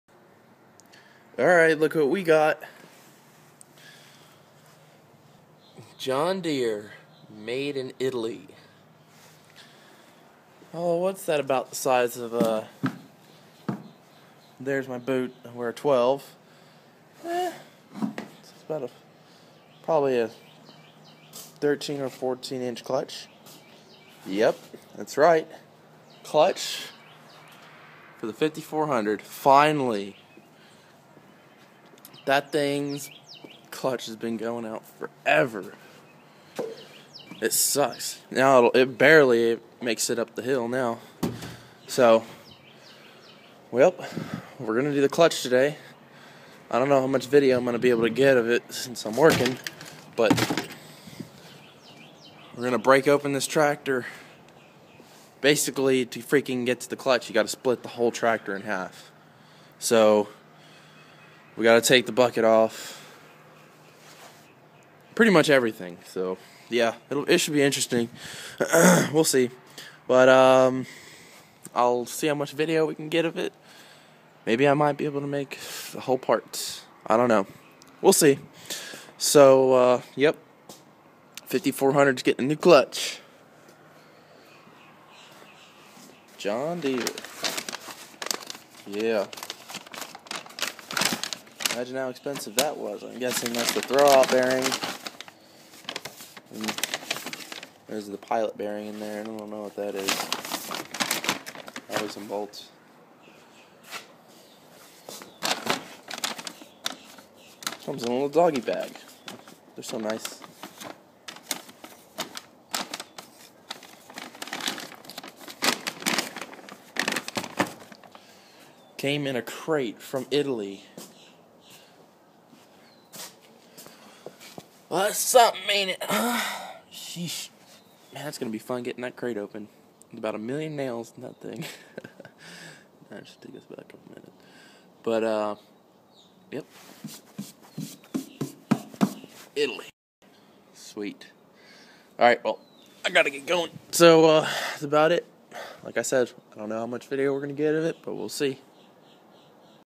5400 john Deere clutch change
Replacing a clutch in a John Deere 5400